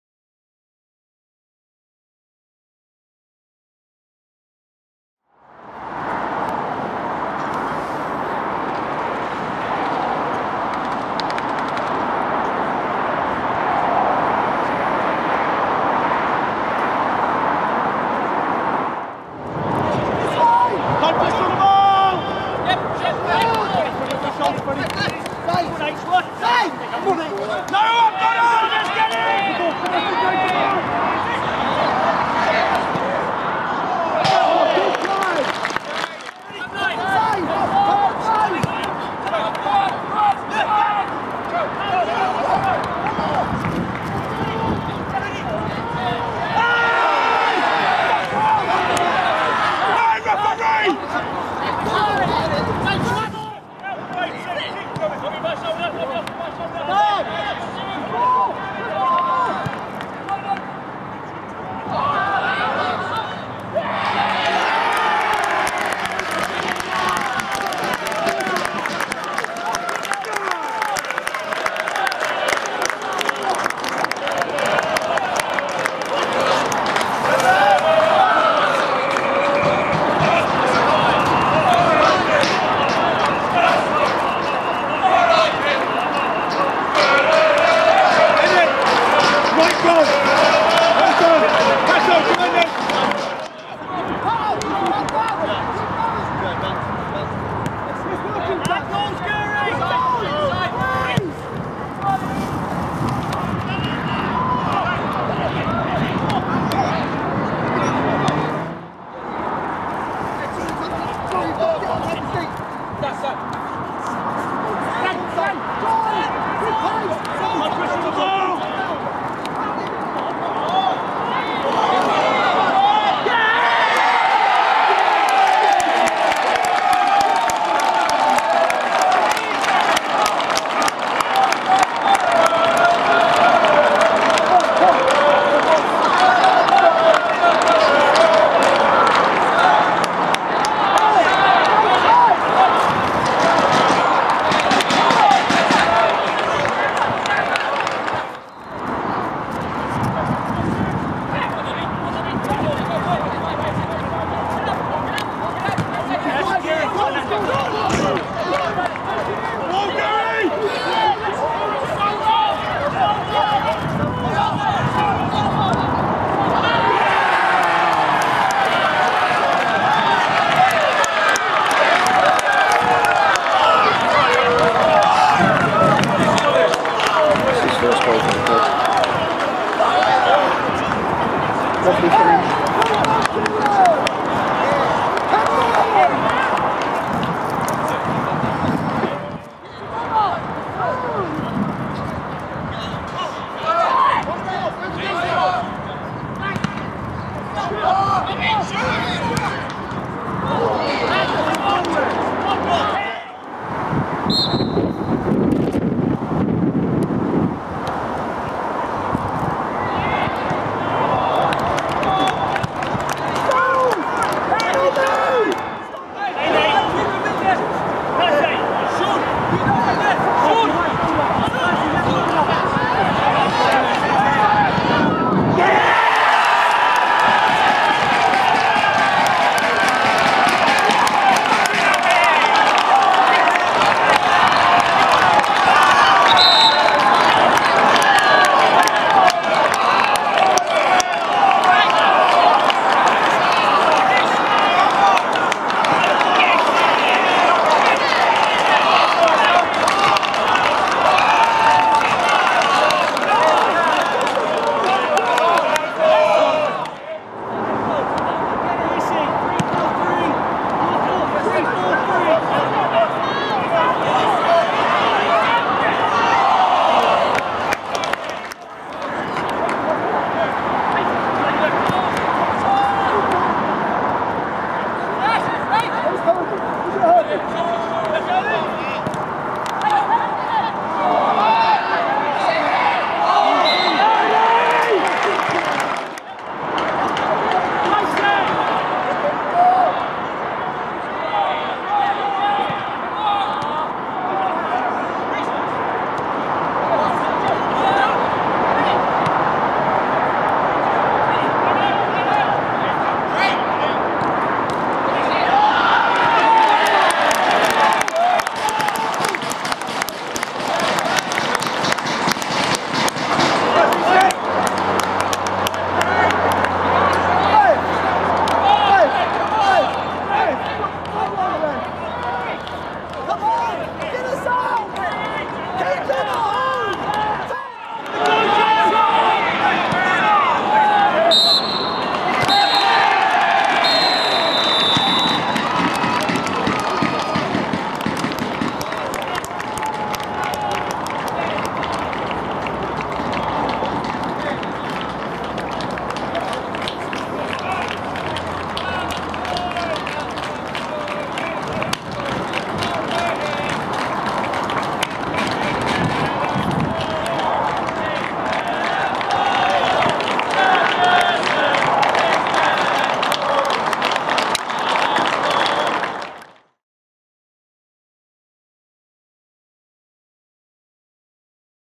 Oxford City v St Albans City FC
Oxford City started their National League South season with a 3-2 home defeat at the hands of St Albans City.
A late consolation from David Pearce failed to reflect the Saints' dominance of the second half.

The game turned on two St Albans goals, one either side of half time, as the visitors came back from a Matt Paterson goal down to take control of the match.

FT Oxford City 2 St Albans City 3